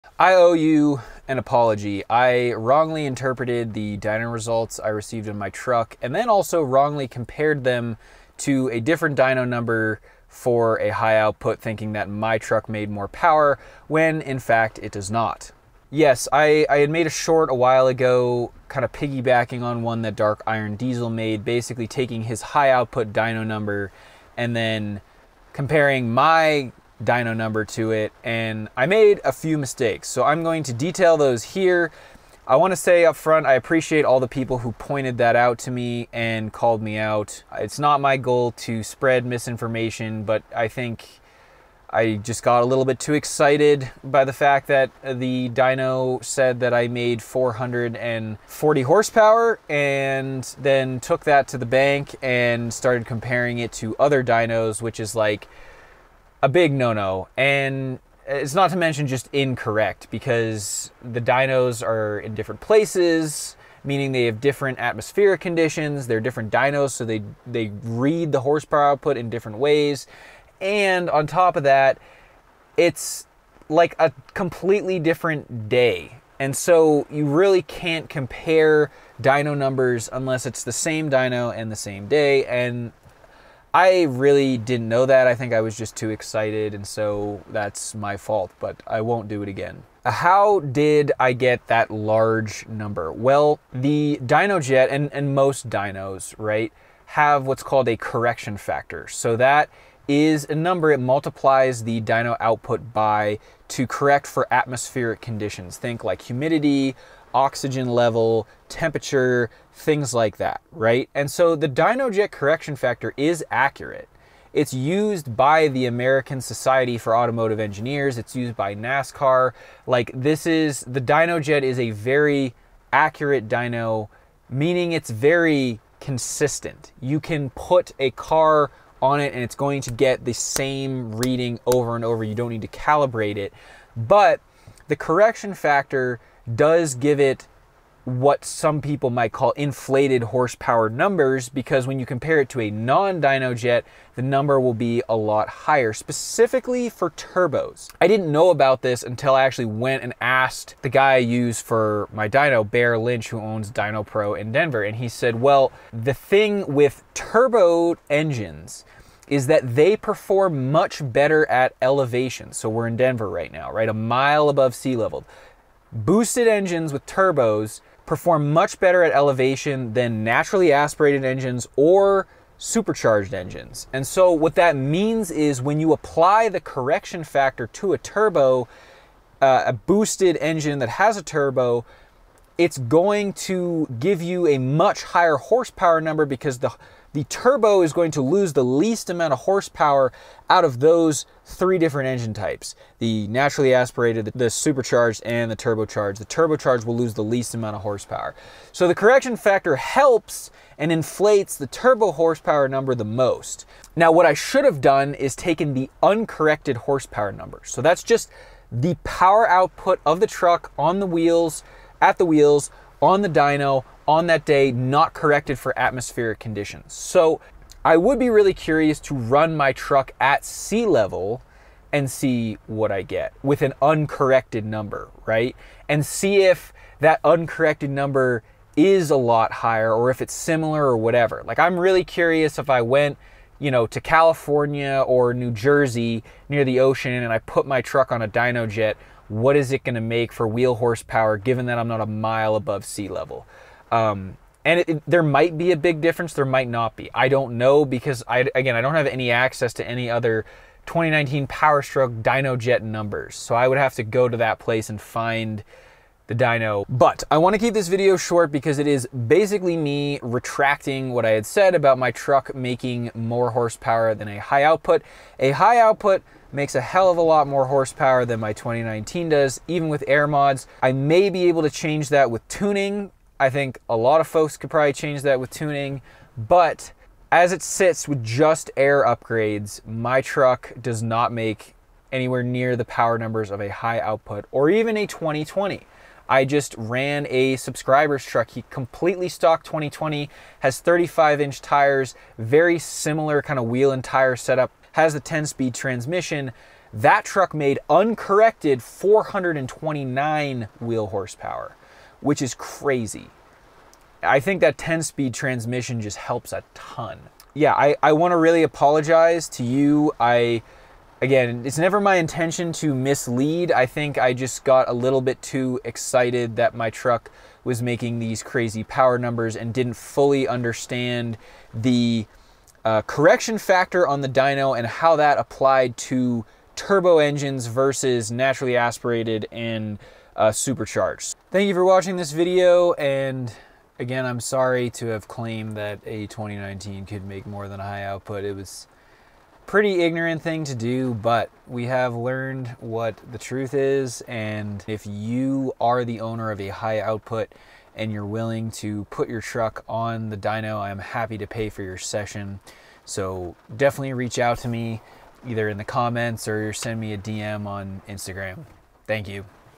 I Was Wrong About My 6.7 PowerStroke Dyno Numbers
I made a mistake when calculating my 6.7 PowerStroke's horsepower and torque!

In this video, I break down how I originally used the corrected dyno numbers instead of the uncorrected numbers while testing my truck at high elevation in Denver.

Since turbo diesel engines perform better at elevation than naturally aspirated engines, they require a much smaller correction factor. I go over how altitude and turbo efficiency impact power output and why my F250’s dyno results were misleading.

Thanks to @darkirondiesel for calling me out.

The tools and gear I use